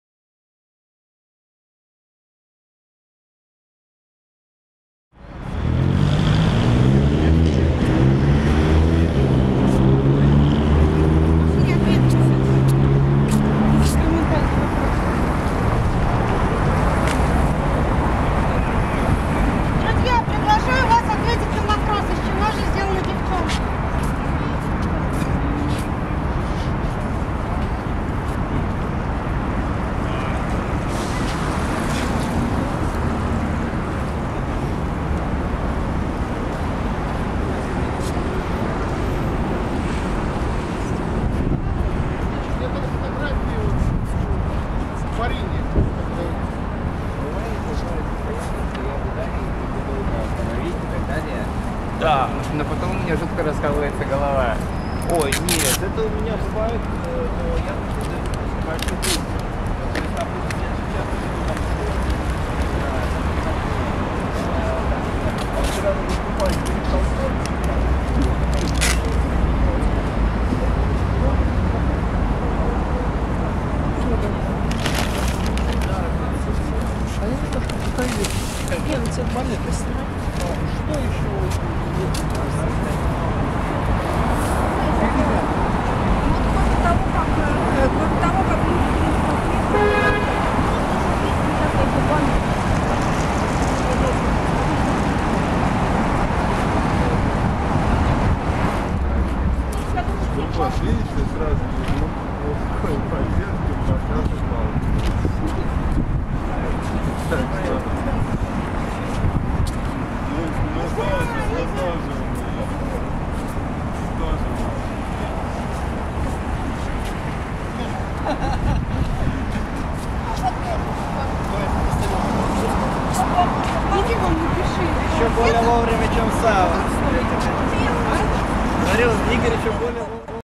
[8 марта] Пикет в Москве
После окончания утреннего митинга в Сокольниках в столице состоялся небольшой пикет, приуроченный к 8 марта. Место проведения — Лермонтовский сквер.

8 марта 2017 г. Снимала Кира.